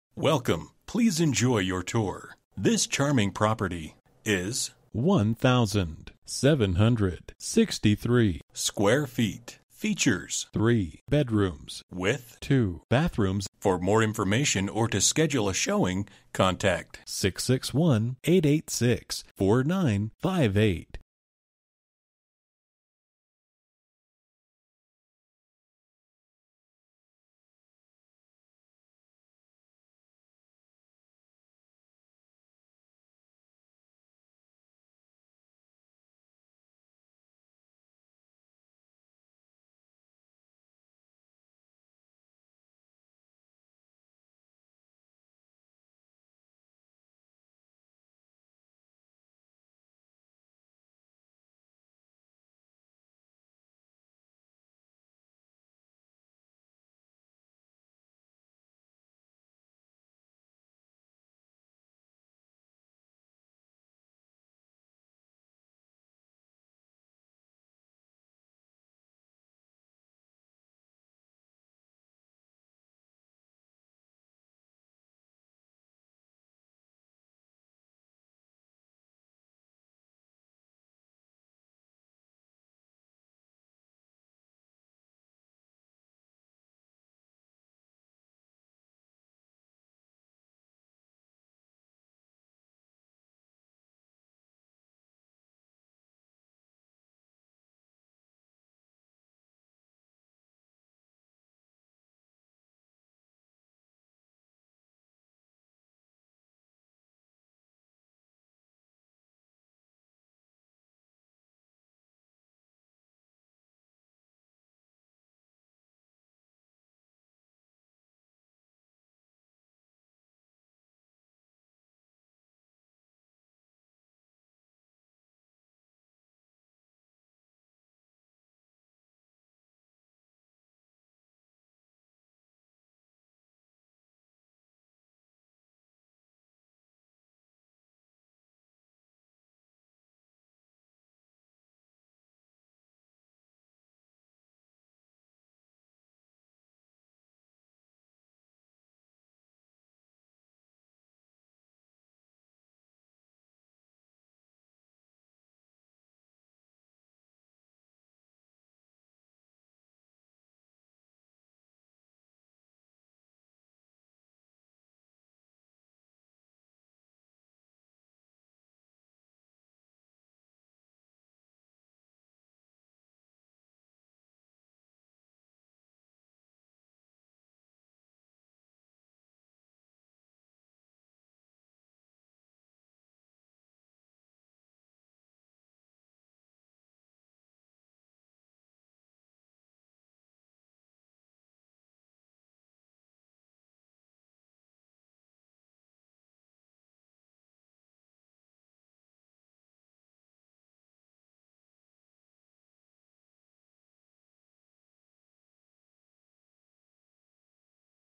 New Residential listing for sale found at 3250 E Avenue H2, Lancaster, CA 93535
This gorgeous ranch style single story offers 3 large bedrooms, 2 full baths and 1,763 square feet of living space. This home is situated on 1.14 acres and is completely fenced and zoned for horses! Upon entering you will notice the open floor plan with a spacious family room accented by a beautiful brick fireplace with wood burning insert. The kitchen has brand new flooring, upgraded appliances, breakfast bar and a lovely bay window looking into the backyard. The master retreat has a large walk-in closet, private ensuite bath and a sliding glass door to an insulated sun room perfect for enjoying a morning cup of coffee while watching the sunrise. Other features include a large indoor laundry room, ceiling fans throughout, high speed internet wiring, and a recently serviced Maytag central heating and air conditioning unit. Outside is a 2 car attached garage, HUGE RV access with a 30 x 12 permitted concrete pad and covering, covered patio, large storage shed and beautiful Joshua Trees!

Bedrooms: 3
Bathrooms: 2
Square Feet: 1763
Price: $340,000

For more information about this property, please contact Jonathan Galier at or . You can also text 6155885 to 67299.